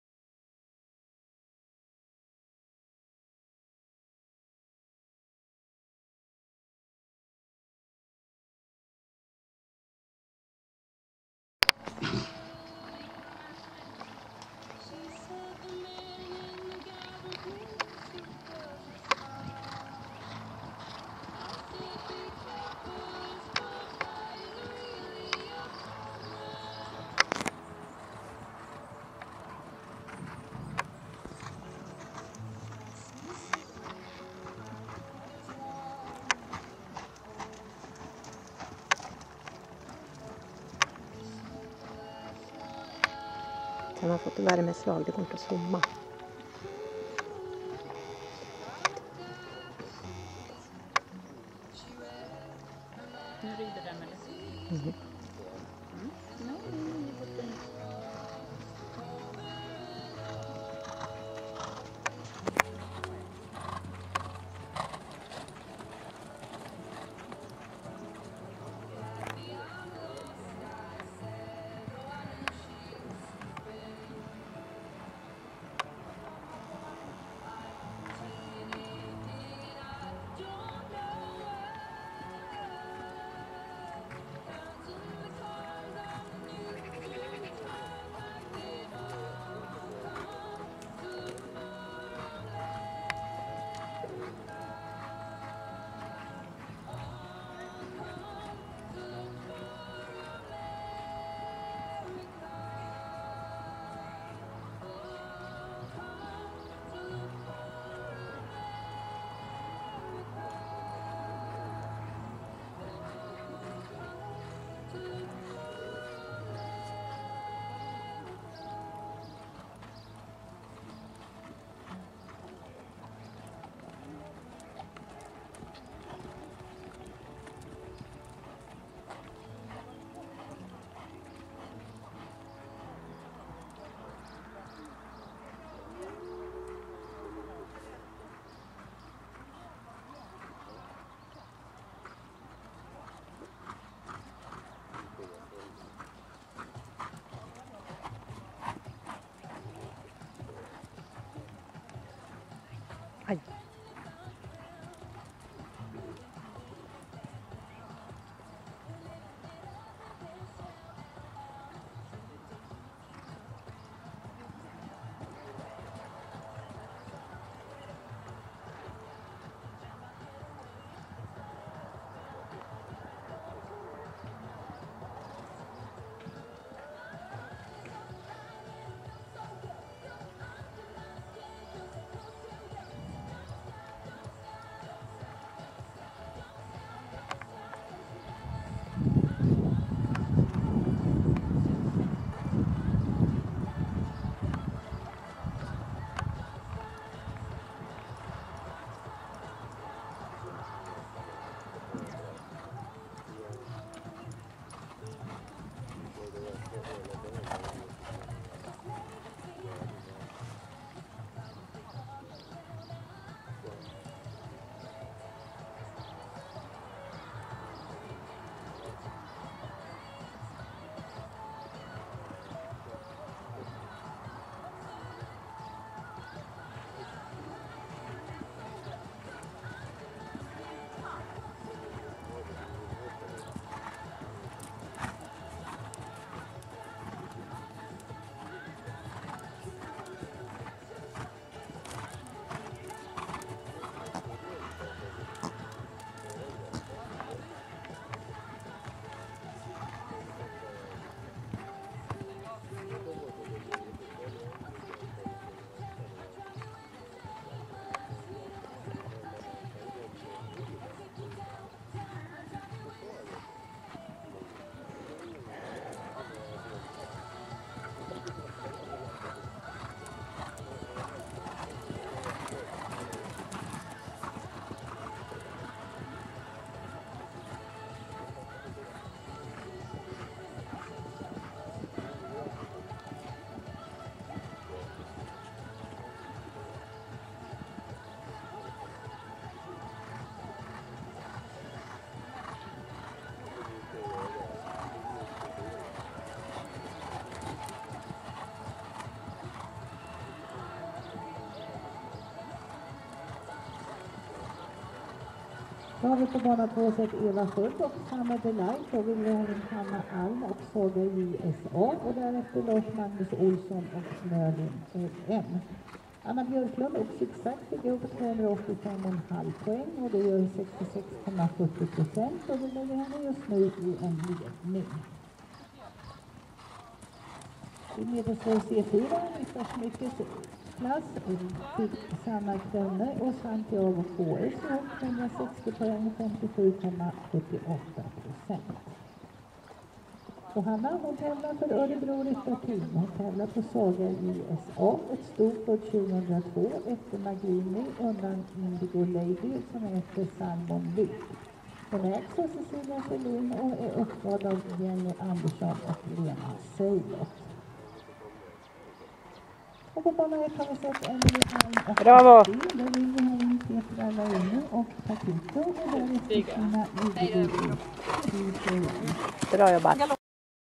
Estée 2015-07-04 MSV C:2 Dyltabruk
Tyvärr en del missar och känslan va inte med oss idag. Bättre visad trav och galopp än gårdagen dock.